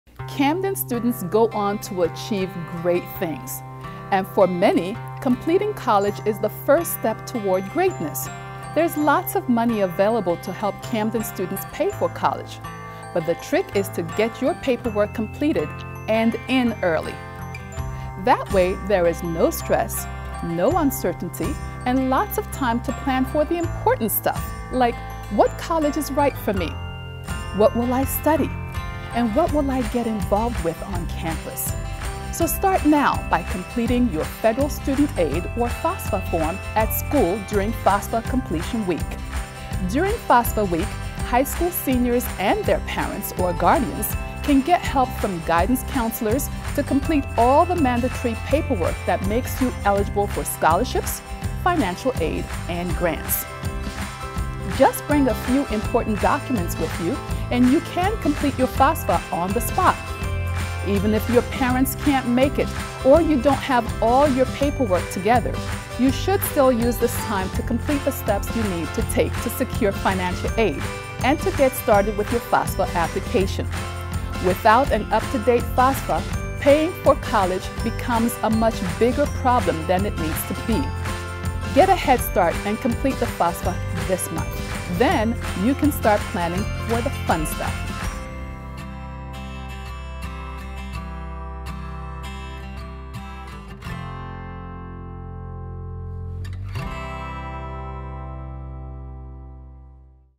FAFSA Week 2016
FAFSA Completion Week is October 31 to November 4. Make an appointment with your high school guidance counselor to fill out your application!